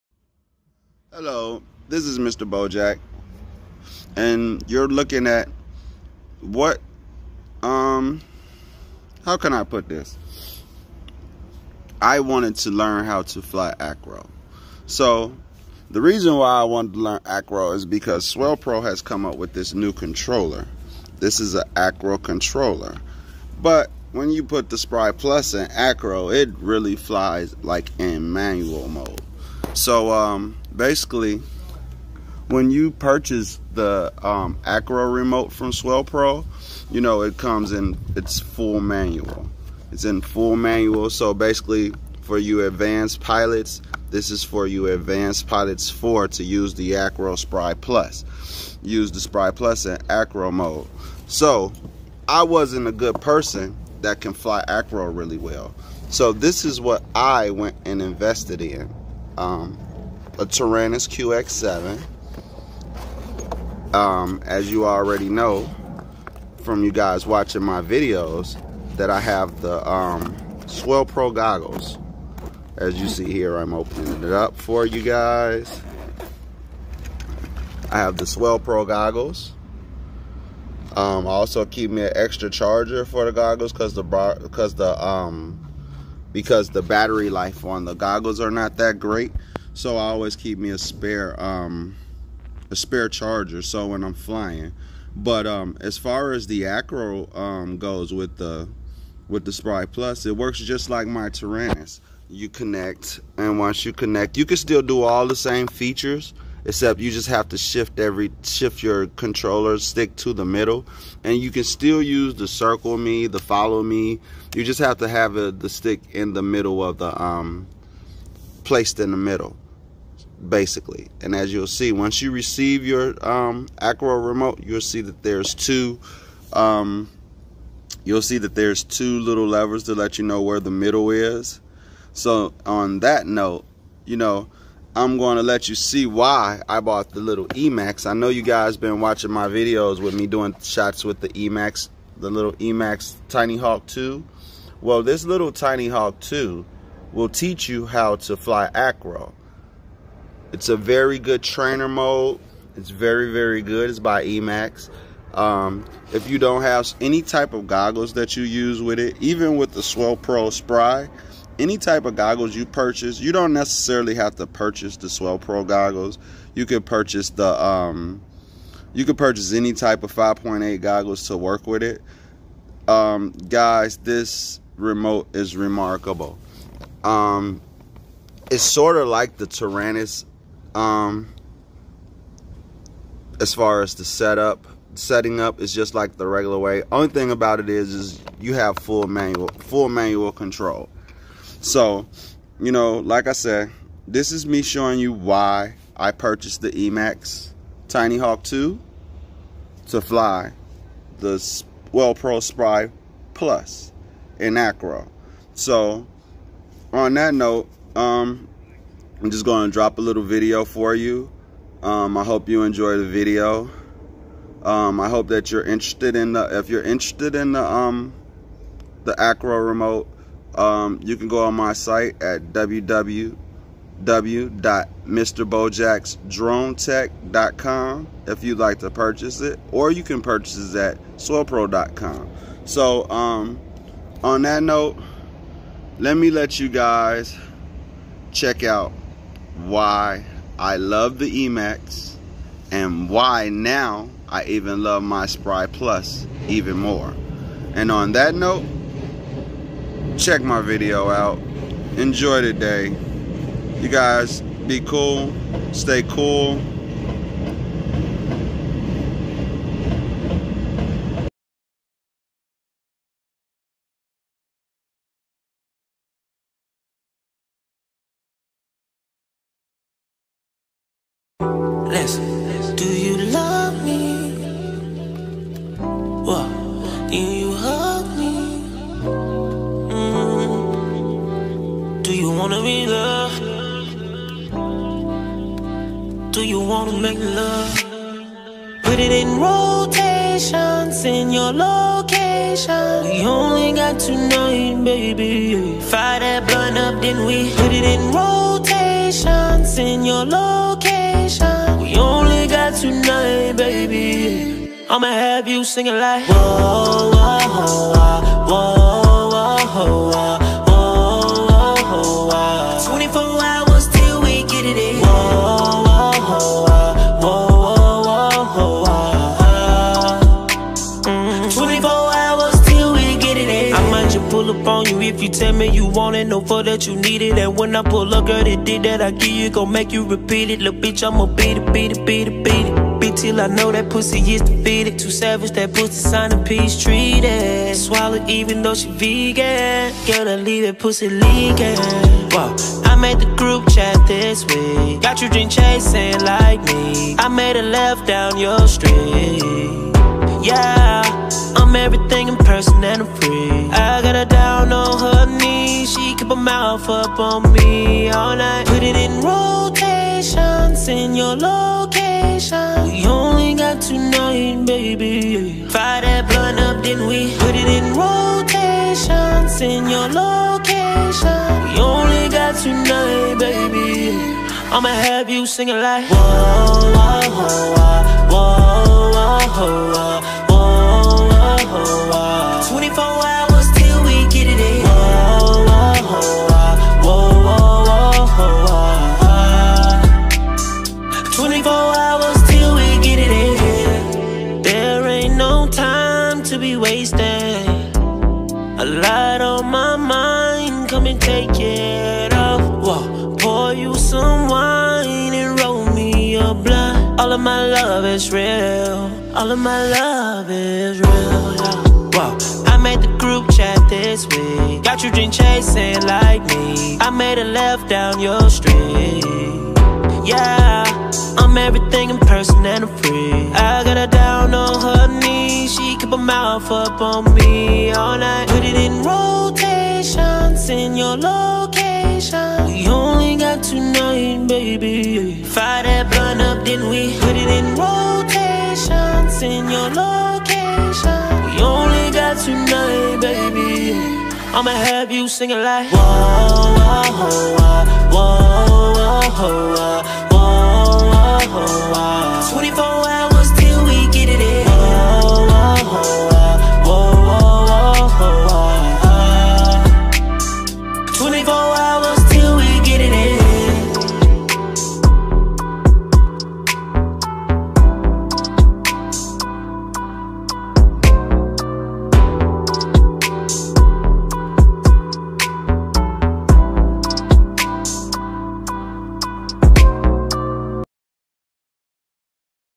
Swellpro Spry+ Acro Remote Tinyhawk 2 Great video at the end
THE NEW SWELLPRO SPRY+ ACRO REMOTE IS SO DIALED IN PERFECT IF YOUR A NEWBIE TO THE ACRO FLYING SO I PURCHASE THE EMAX TINYHAWK 2 TO TEACH ME HOW TO FLY ACRO THIS IS A GREAT FLYER

TO PURCHASE A SWELLPRO ACRO REMOTE

TO PURCHASE A TINYHAWK 2